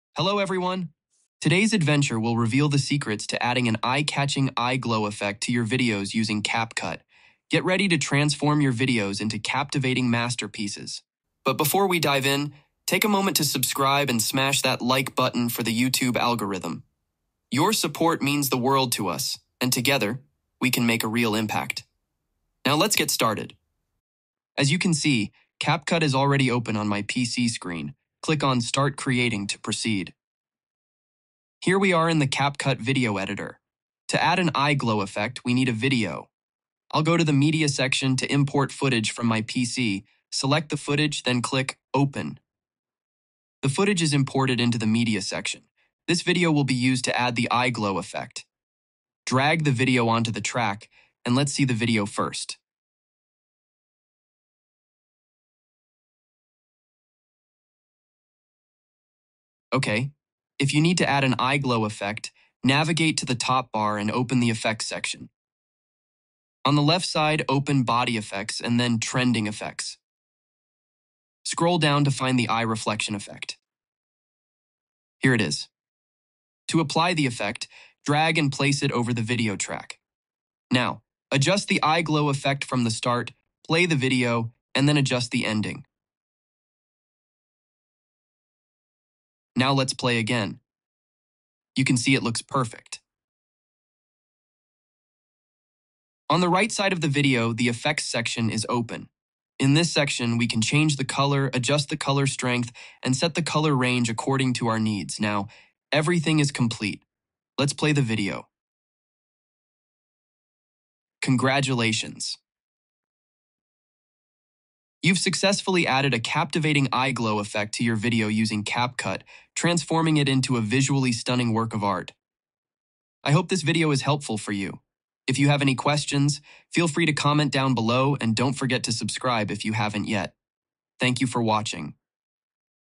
How To Add Eye Glow Effect in Capcut PC version [Easy Toturial]
Welcome to our video tutorial on transforming your videos into captivating masterpieces with the mesmerizing "Glowing Eyes Effect" using CapCut!

In this step-by-step guide, we'll show you exactly how to add that eye-catching glow to your videos, elevating them to a whole new level of visual appeal. Whether you're a seasoned video editor or a beginner, our easy-to-follow instructions will make the process a breeze.

🎥 What You'll Learn:

- Opening CapCut like a Pro 🚀
- Importing Footage for the Perfect Glow ✨
- Applying the Eye Reflection Effect 💫
- Customizing Colors and Strength 🌈
- Final Touches for a Stunning Finish 🎨

🔥 Why Glowing Eyes?

The glowing eyes effect adds an enchanting touch to your videos, creating an otherworldly and visually stunning atmosphere. Perfect for storytelling, music videos, or simply adding that wow factor to your content.

💡 Pro Tips:

Experiment with different colors and strengths to find your unique style.
Use the effect strategically for emphasis and storytelling.
Share your glowing creations on social media and stand out from the crowd.